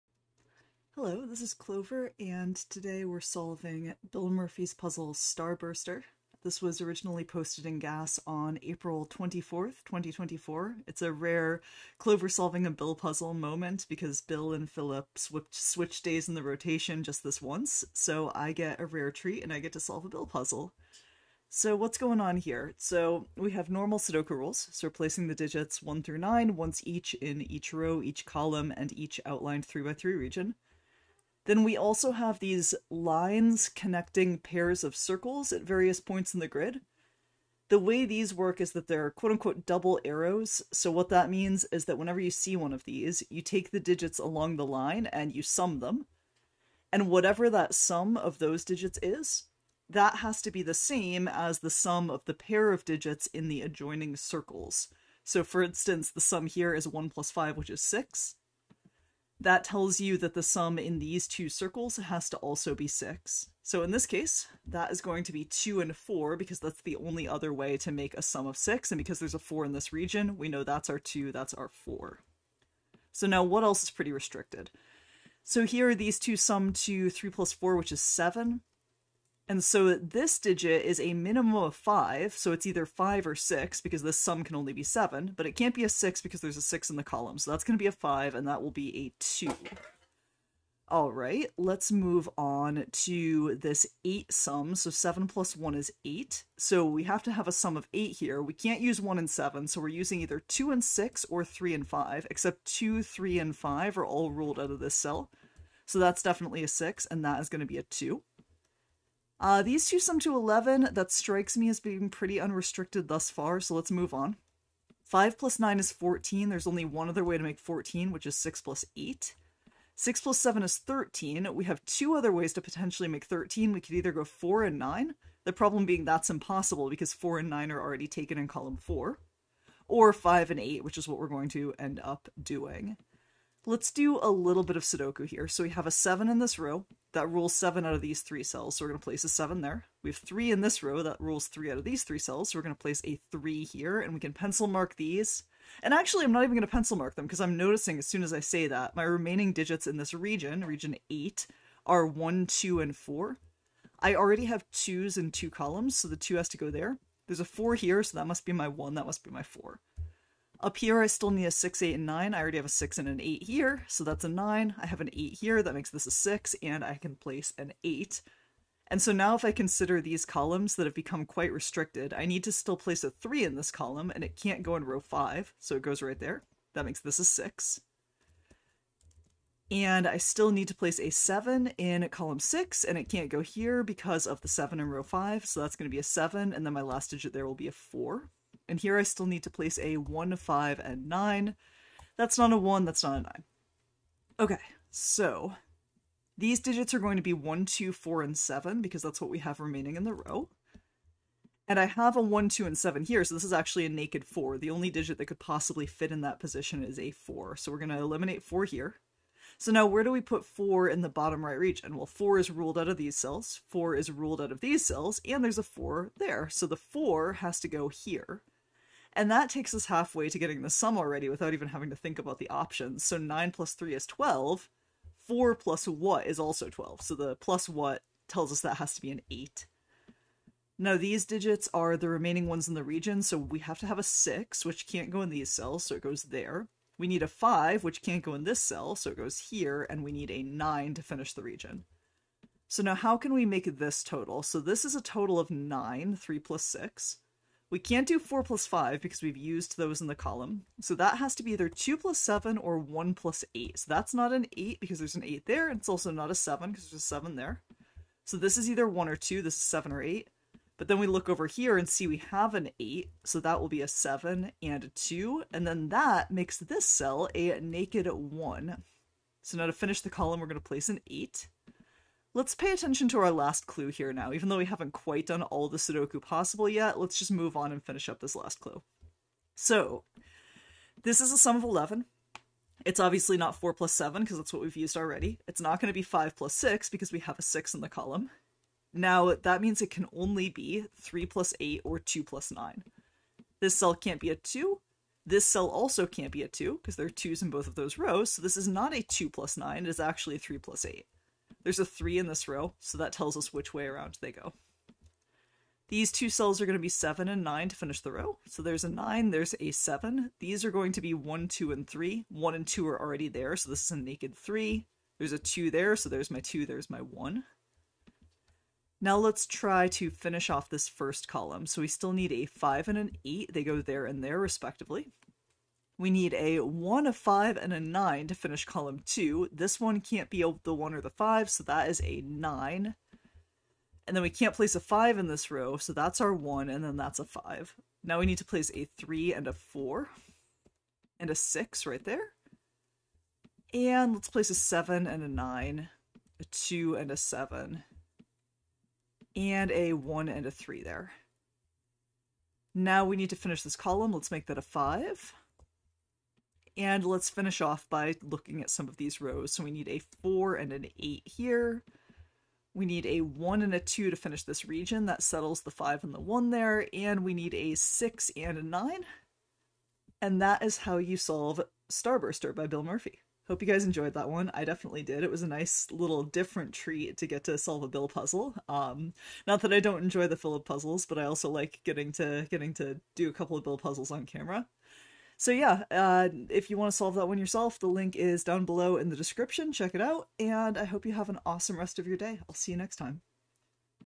GAS Sudoku Walkthrough - Starburster by Bill Murphy (2024-04-24)
Clover solves Bill's sudoku "Starburster", originally posted on April 24, 2024.

RULES:
Normal sudoku rules apply. Digits along lines must have the same sum as the digits in the circles at each end.

HAT TIMES:
7:30/13:00